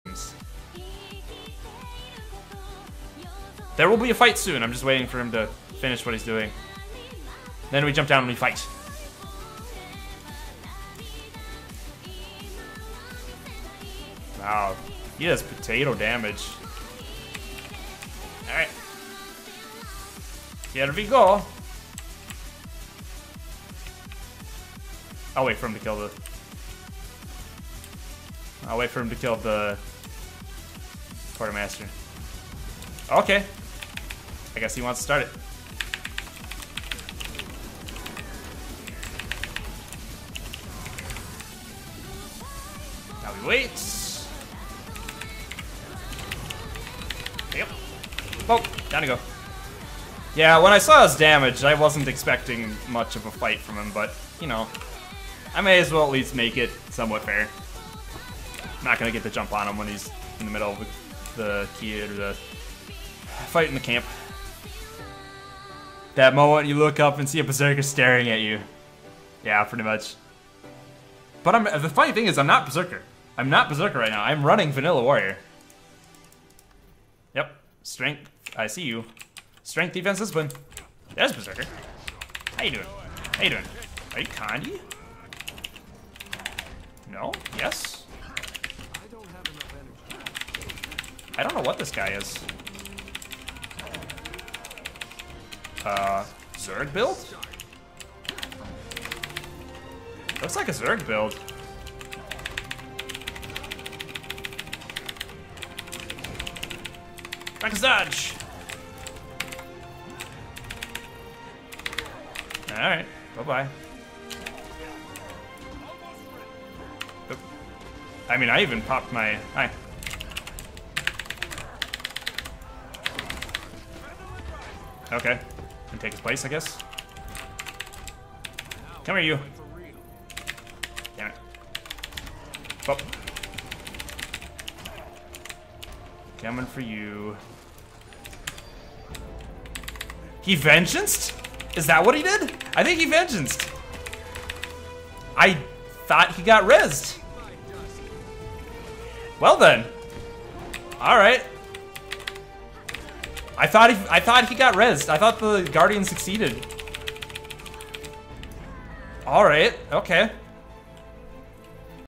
5/16/17 Warrior Changes Test #3
Tested out the changes to axe and Shield Bash using base warrior on stream. Second camp test.